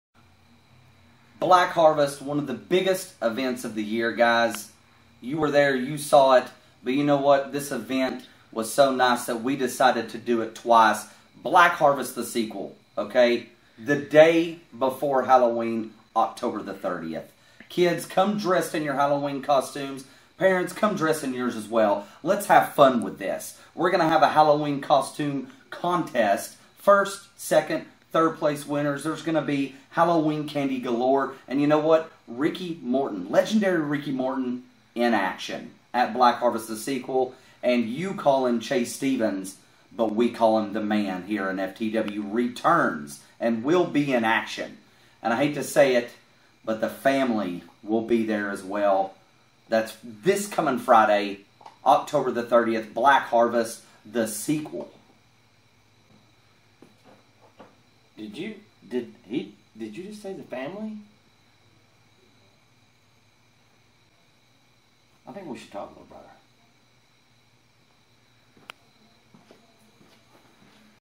Issac Cain Speaks on Black Harvest The Sequel News!
Black Harvest The Sequel News- MUST SEE EVENT. Halloween costume contest. Ricky Morton and the RETURN of CHASE STEVENS. Be there to see chase stevens make history at BH THE SEQUEL! This Friday 1004 Parkway Sevierville tn. RV Resort Conference center.